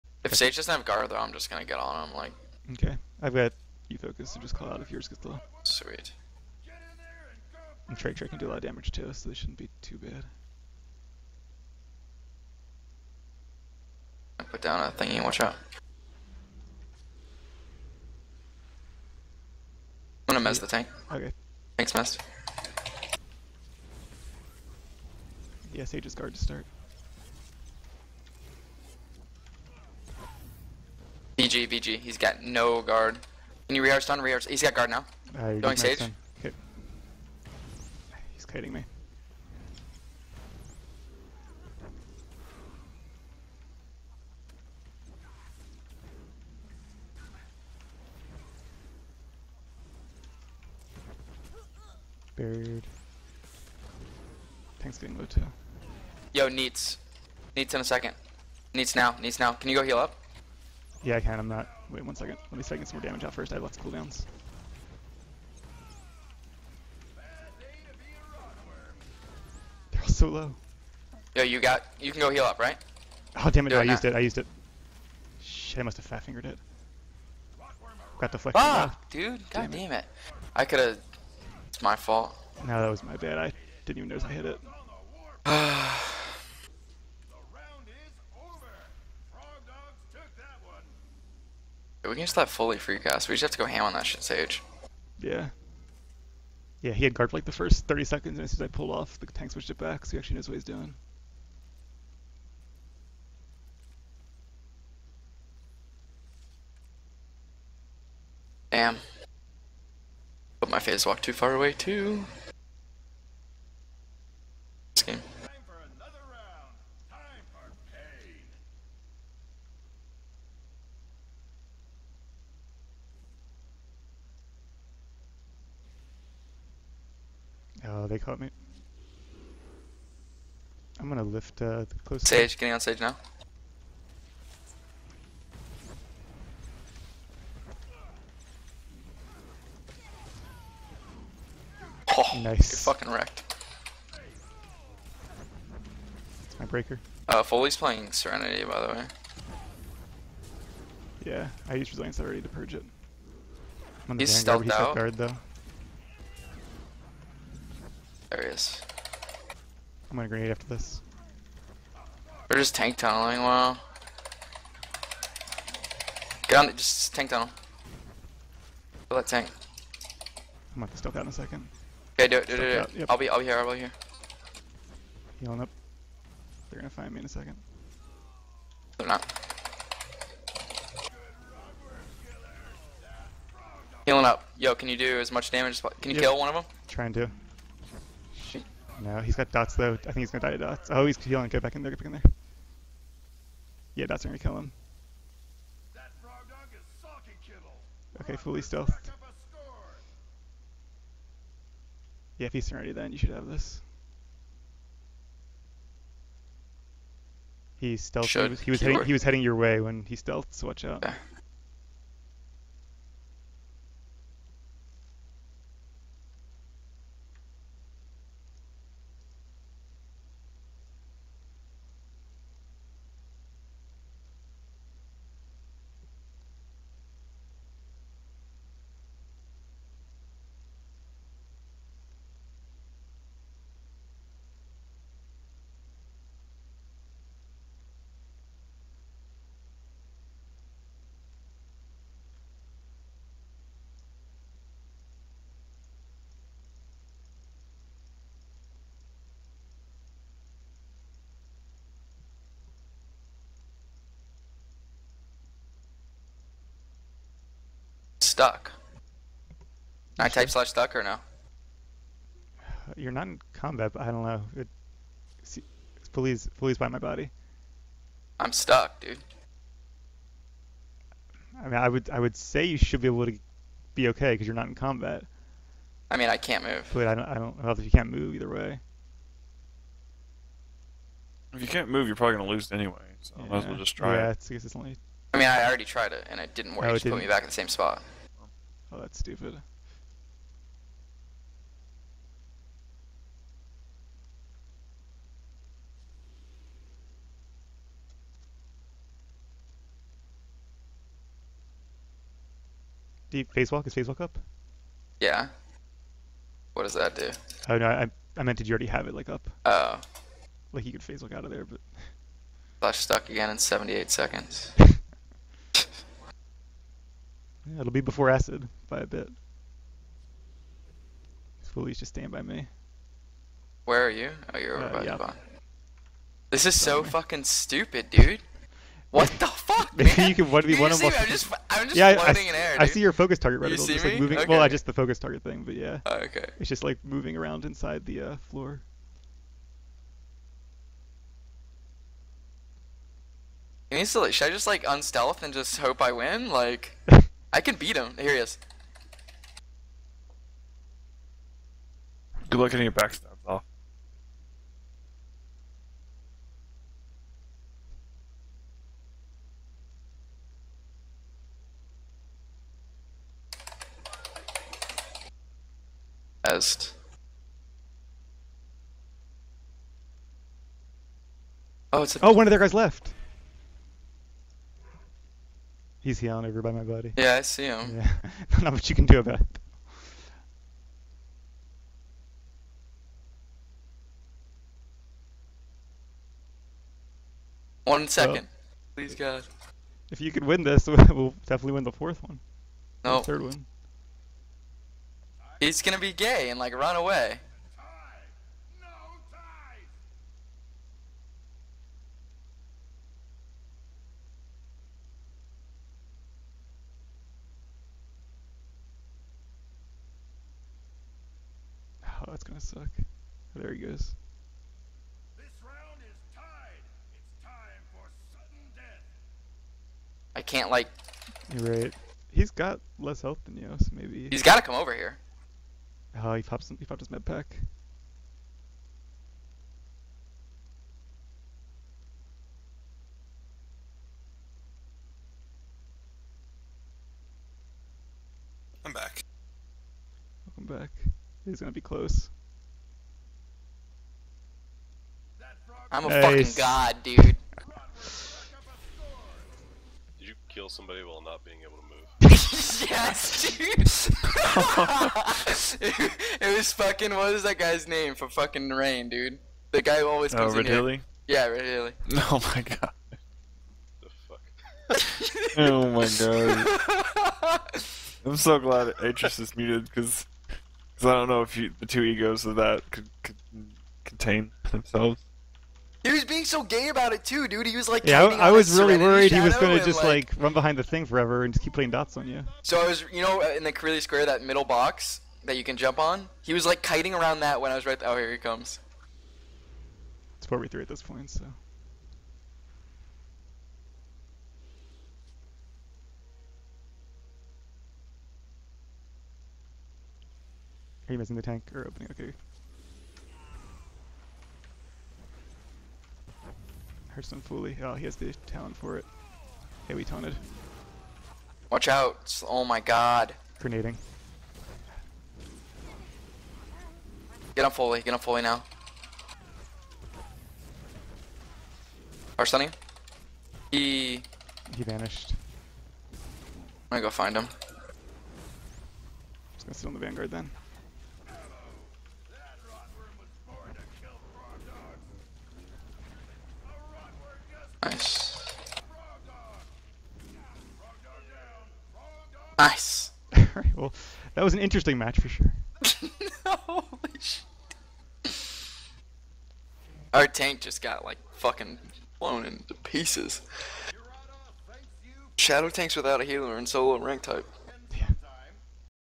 Ranked Season 5: Liavara (Match 138) - Fullee vs. Jidoh vs. Terrain
"I'm a fucking god, dude."   This was definitely a weird match to end the season on, but I'll take it.  Poor Lia never made it back up to the 1540 she had at match 52, but she had all the prizes from group ranked anyway, so I didn't do as many solos on her this season.